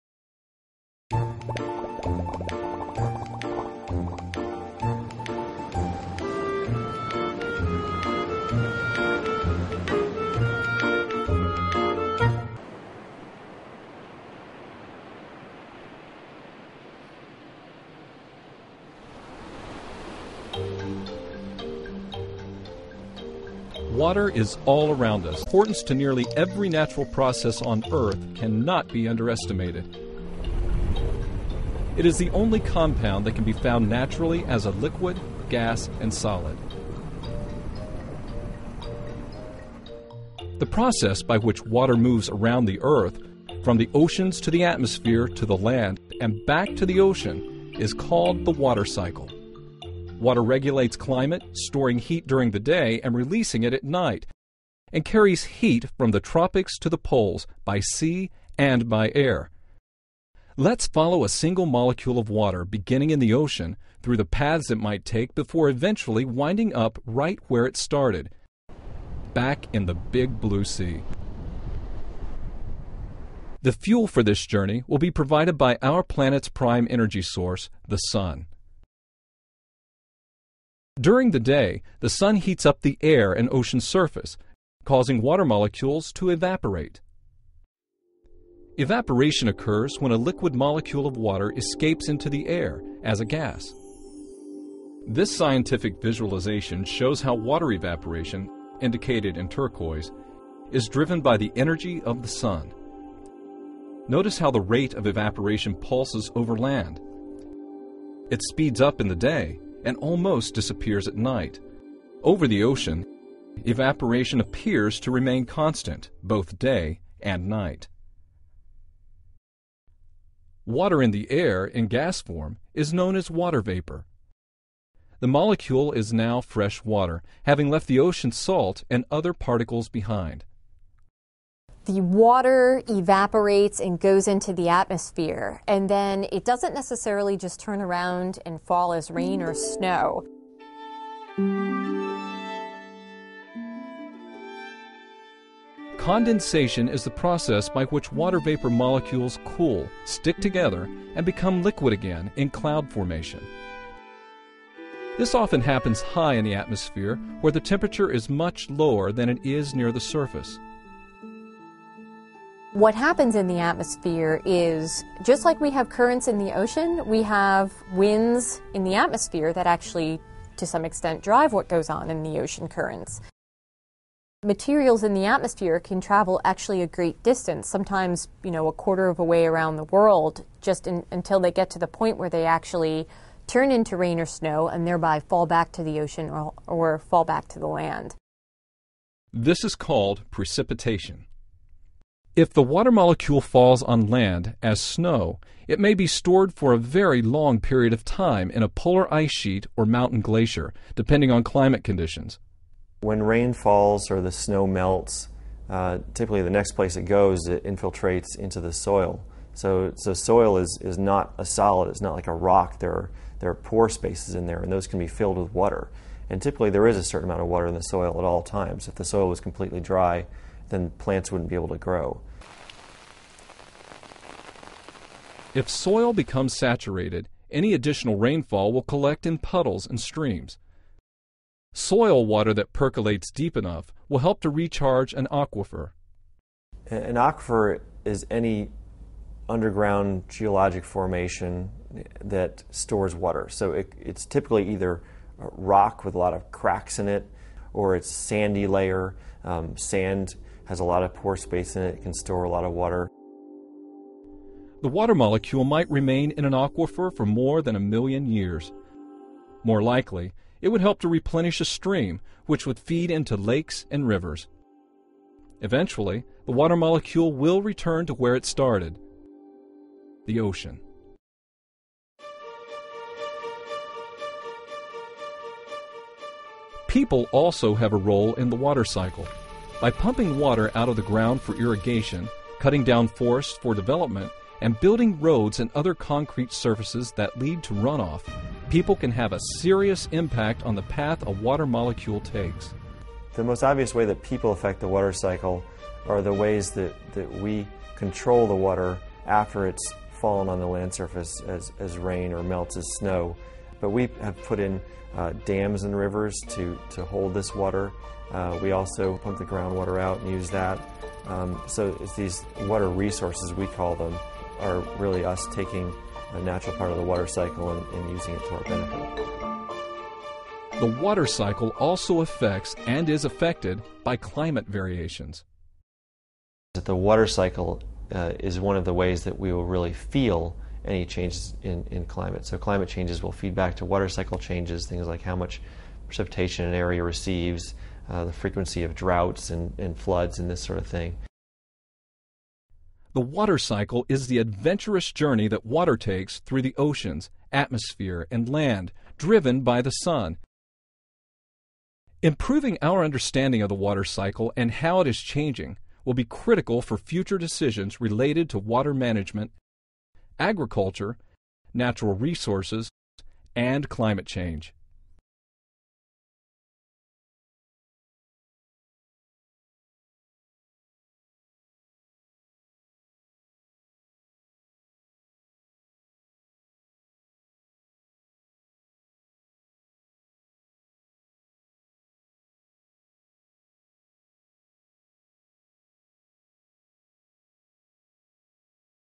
מים בכל מקום
להסבר מלא כנסו אל זהו סרטון מתוך המדור 'מאגר מדע' של האתר דוידסון און-ליין. דוידסון און-ליין הוא אתר חינוכי מבית מכון דוידסון לחינוך מדעי - הזרוע החינוכית של מכון ויצמן למדע.

סרטון זה מסביר אודות מחזור המים והשפעת האדם עליו.

 הסרטון הופק בידי NASA.